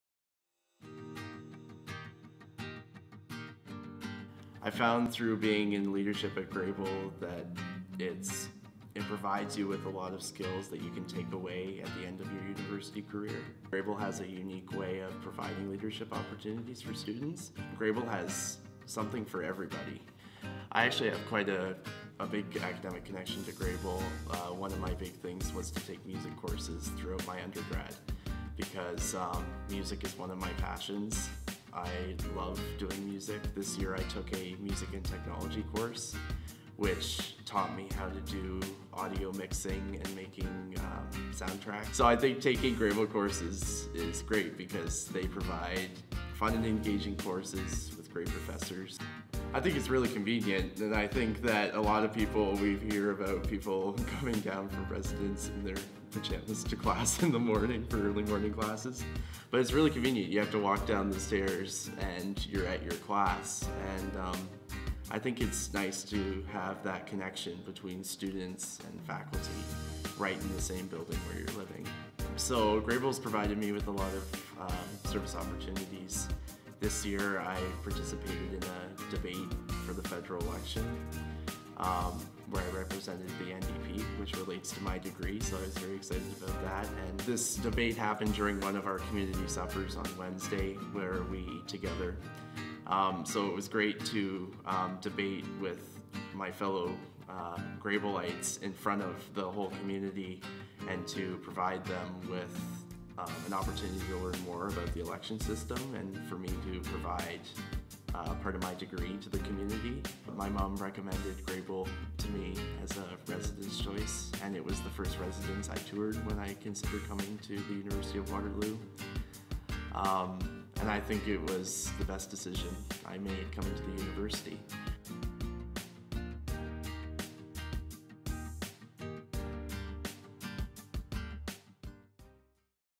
Living at Grebel - Matt
Matt shares about Grebel classes and interesting opportunities.

Song:
Danny E.B The Producer.